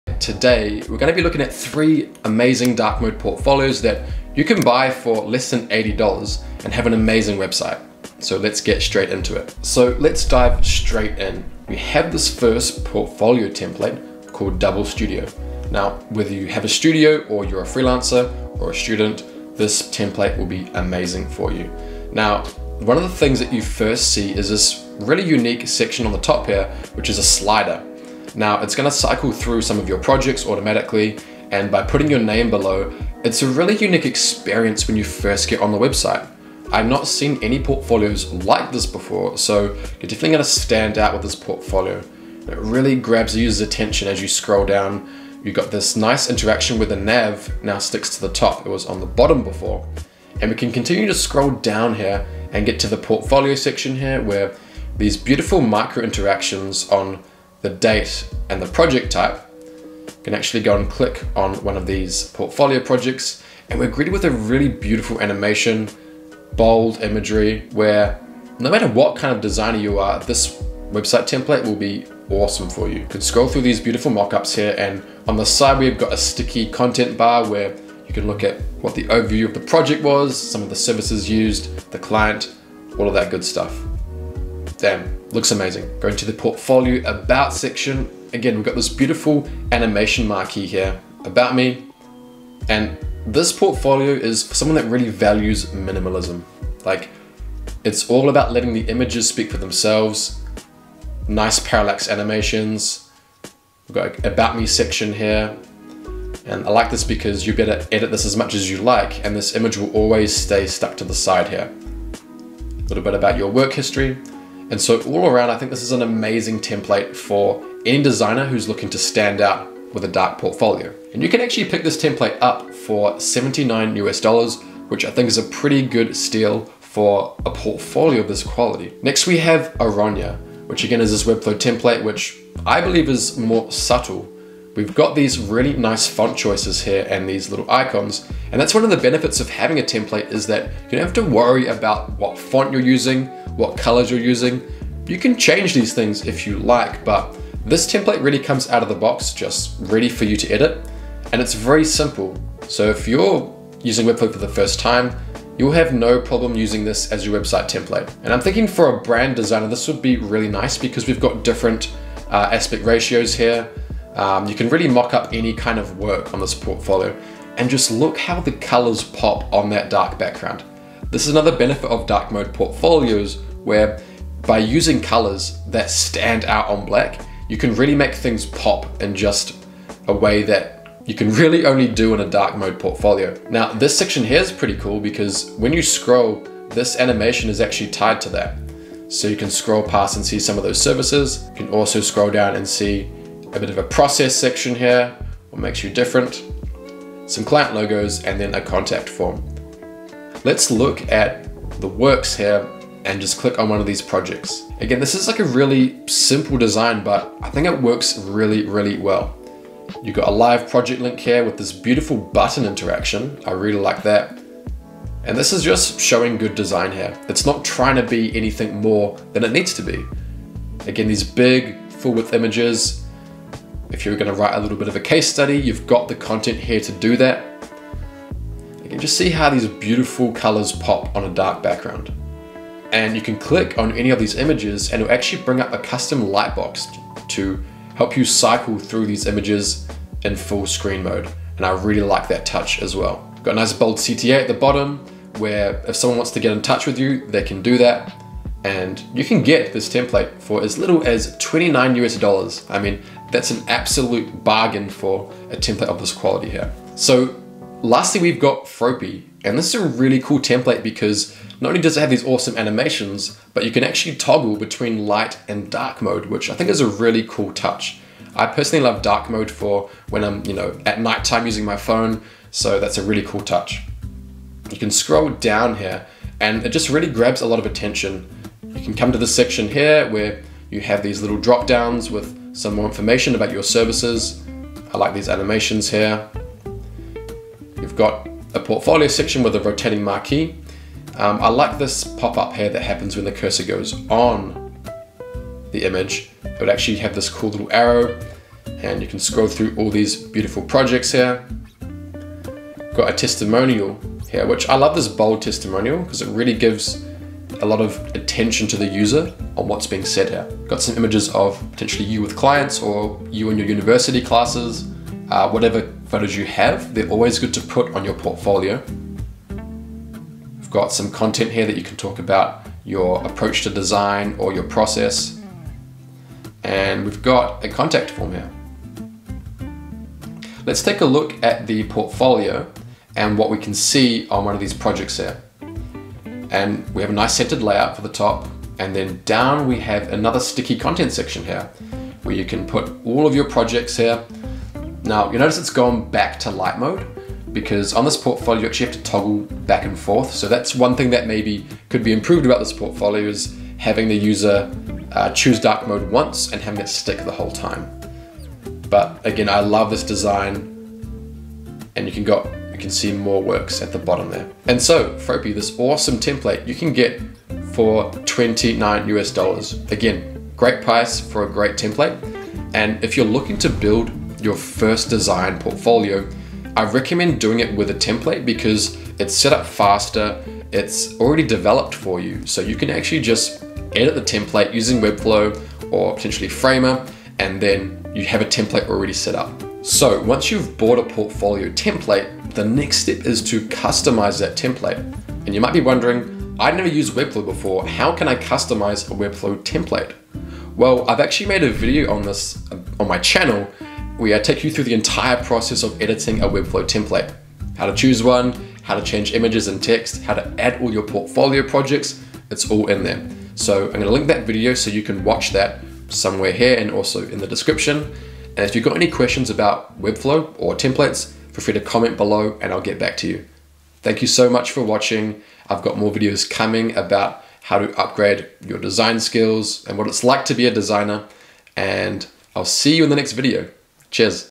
3 AWESOME Dark Mode Portfolios for Designers in 2024
Hi I'm Finn, I'm a UX designer at Antler.

Having a dark mode portfolio is a great way to stand out as a designer.

Here are three portfolios templates on Webflow to get you started!

Chapters

00:00 Intro
00:25 Template #1
02:24 Template #2
04:32 Template #3